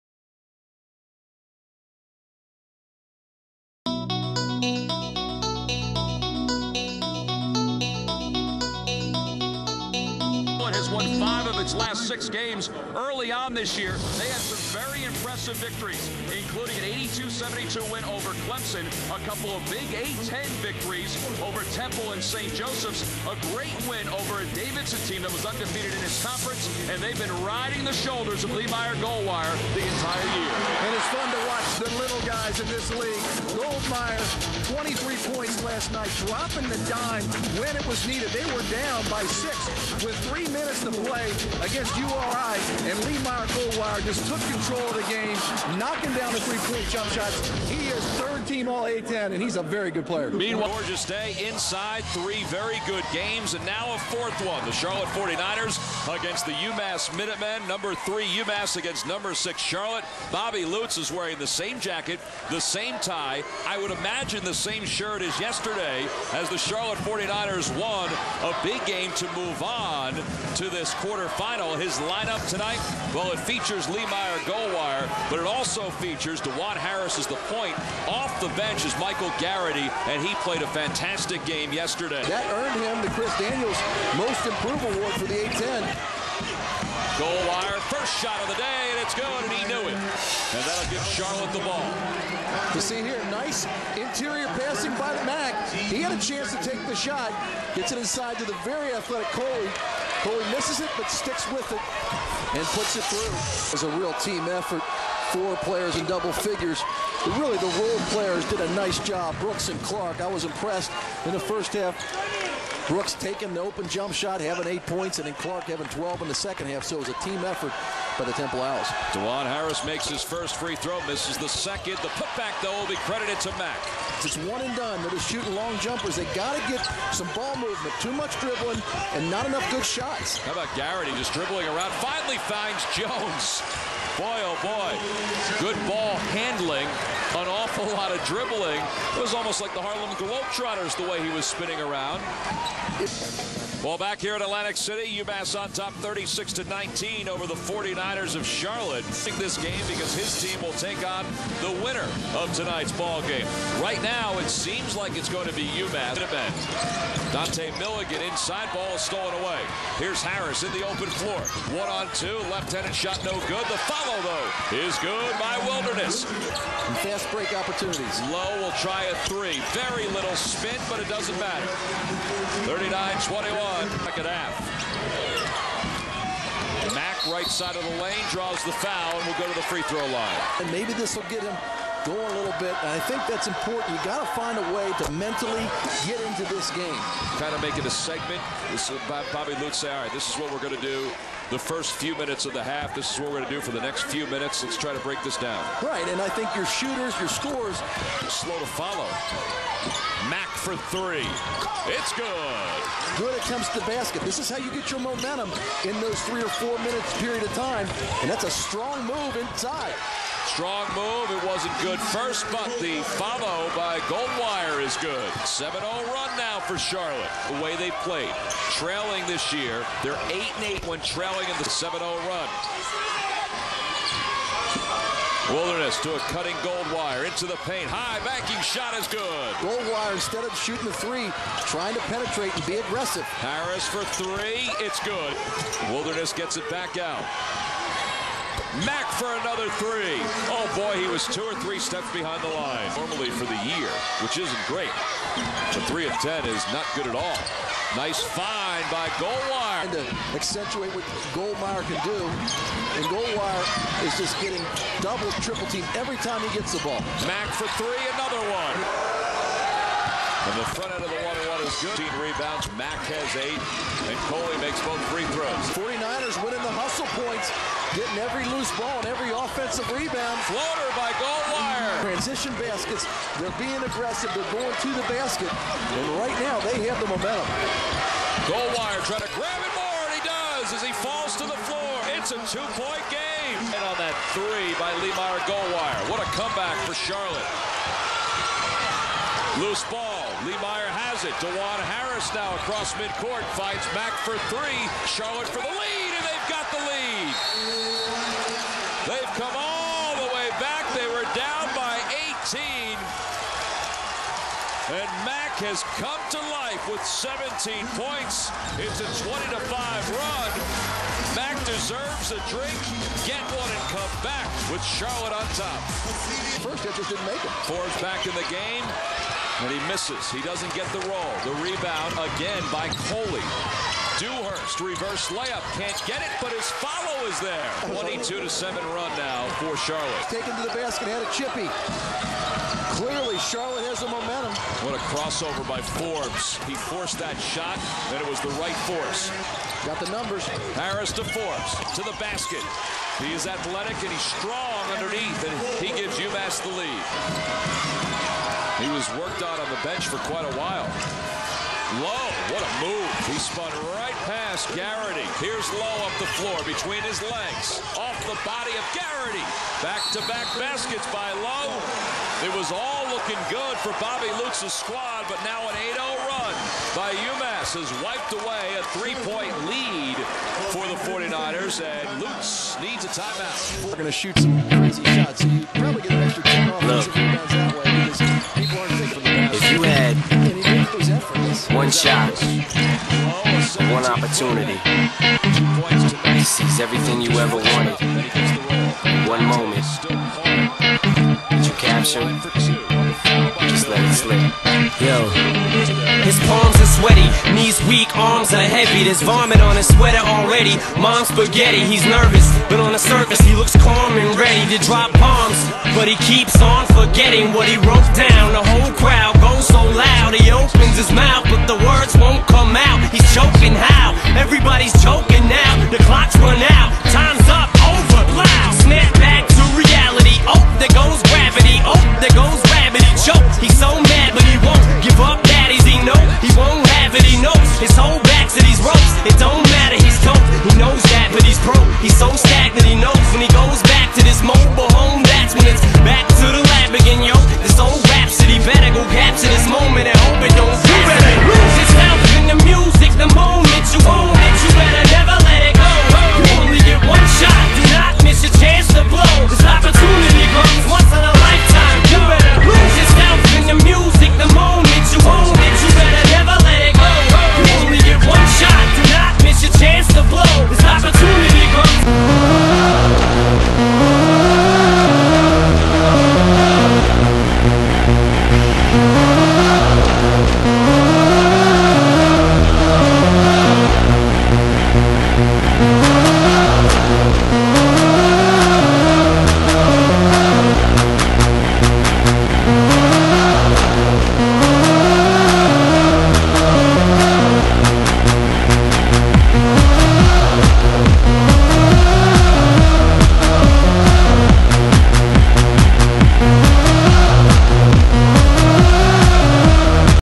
The Charlotte 49ers beat The UMass Minutemen
The Charlotte 49ers beat The UMass Minutemen on March 13, 2008 in the quarter-final game of the 2008 Atlantic 10 Men's Championship at Boardwalk Hall in Atlantic City, New Jersey by a score of 69-65.